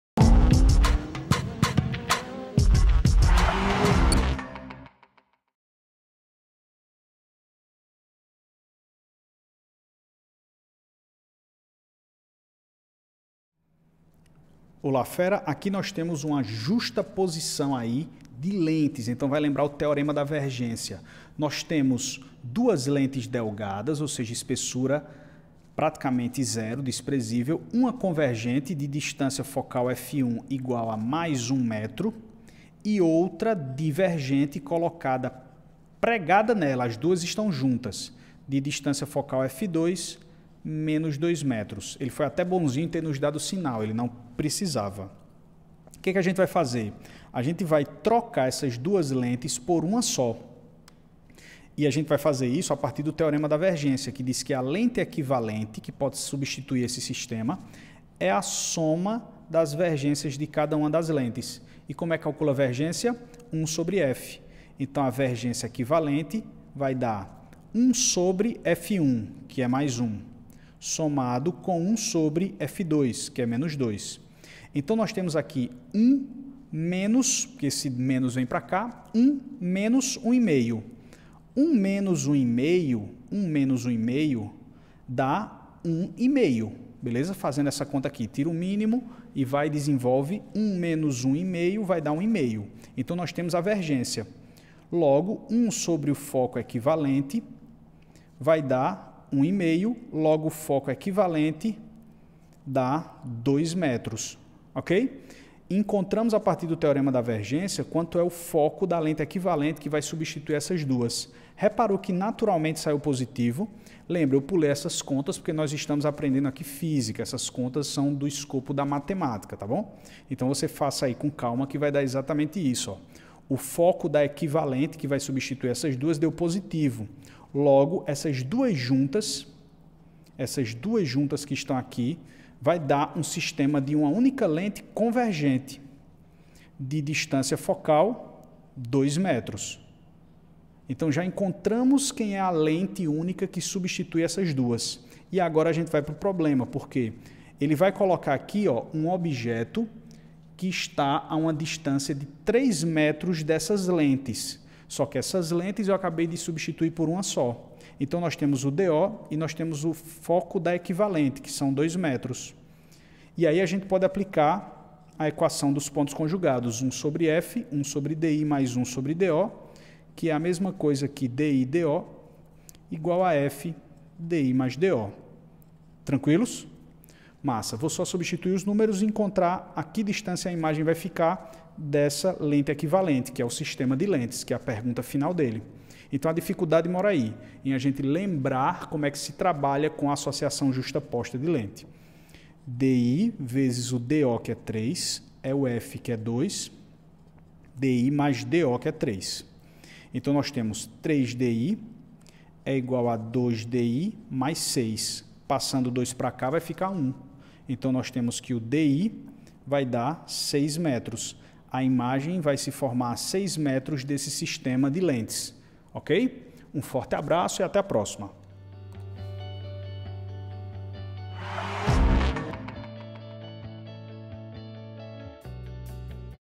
23 - Lentes Esféricas
(Vunesp-SP) Duas lentes delgadas, uma convergente e outra divergente, com distâncias focais respectivamente iguais a 1 m e –2 m, encontram-se justapostas. Um objeto é colocado a 3 m das lentes. Desprezando a espessura do sistema de lentes, determine a distância entre a imagem e esse sistema.